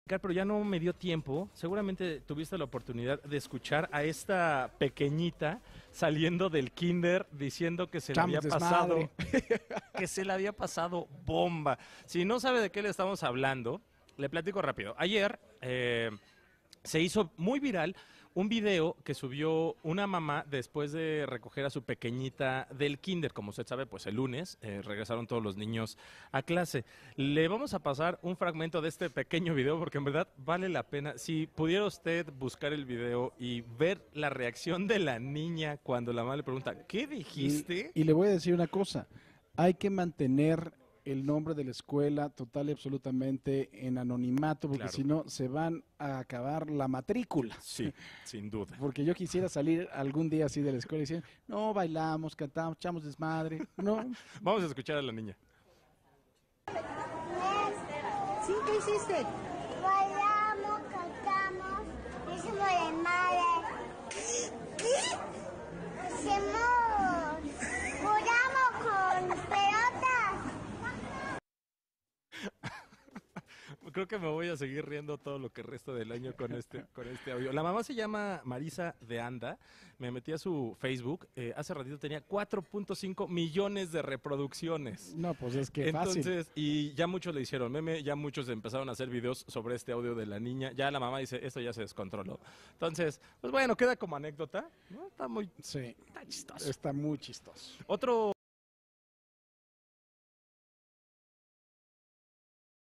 Bailamos, cantamos, hicimos desmad..., así fue el primer día de clases en el kinder
La reacción de una alumna de kínder enterneció a los usuarios de redes sociales, luego de que la pequeña confesó a su mamá que en su primer día de clases ‘bailó, cantó e hizo desmadre’